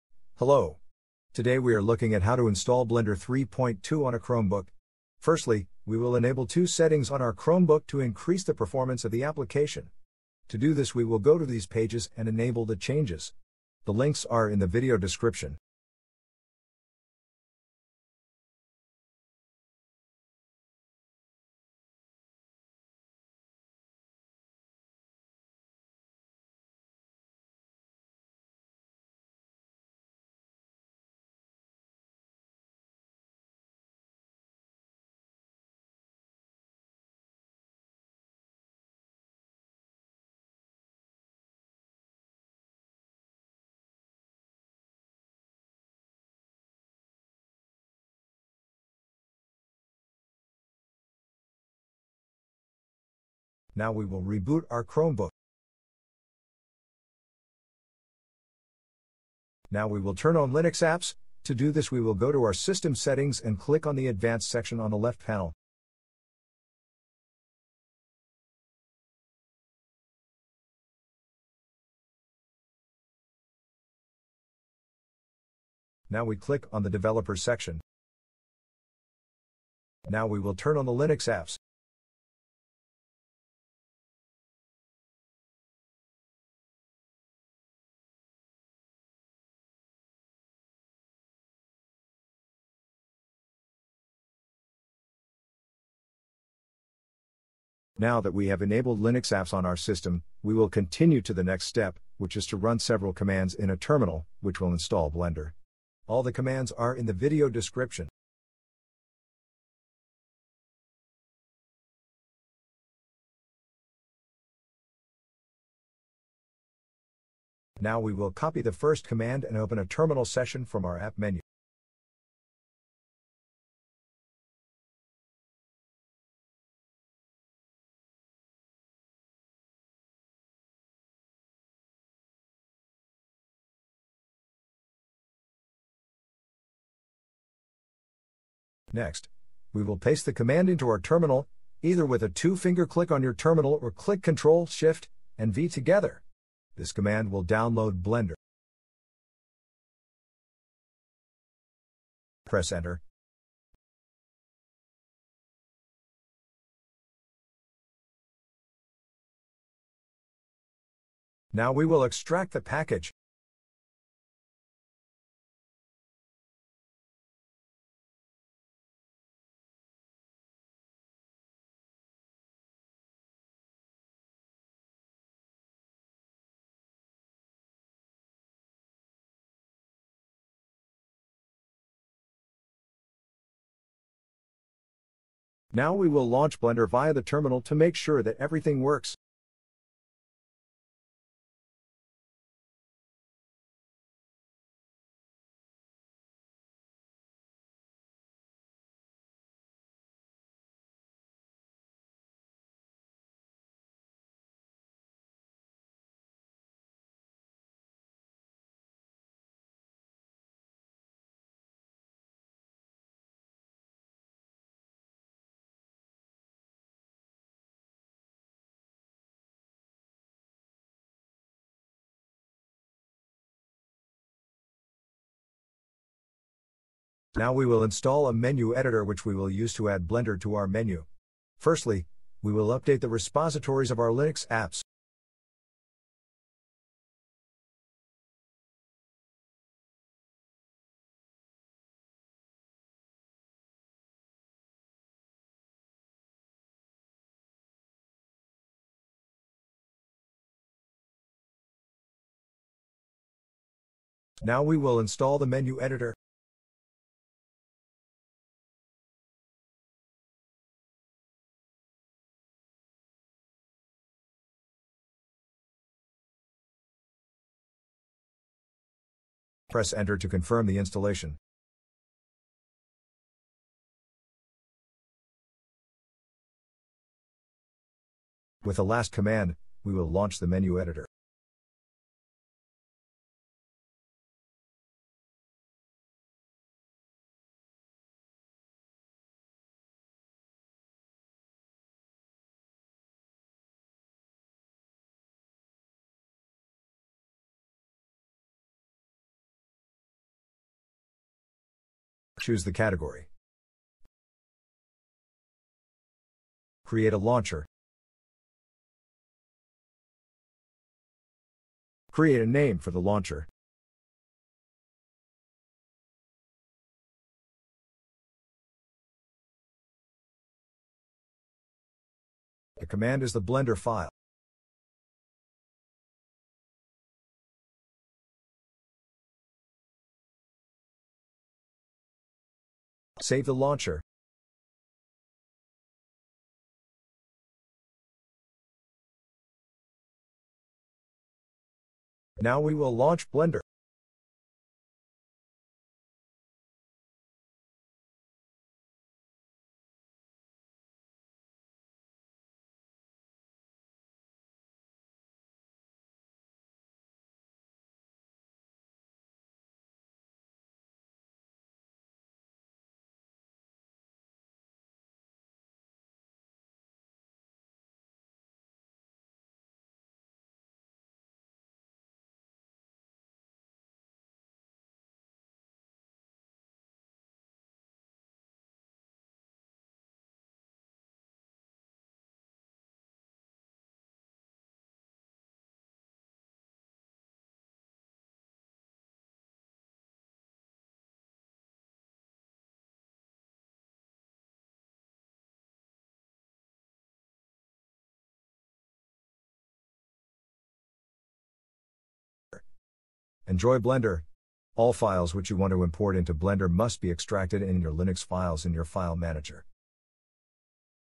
How to install Blender 3.2.0 on a Chromebook
In this video, we are looking at how to install Blender 3.2.0 on a Chromebook.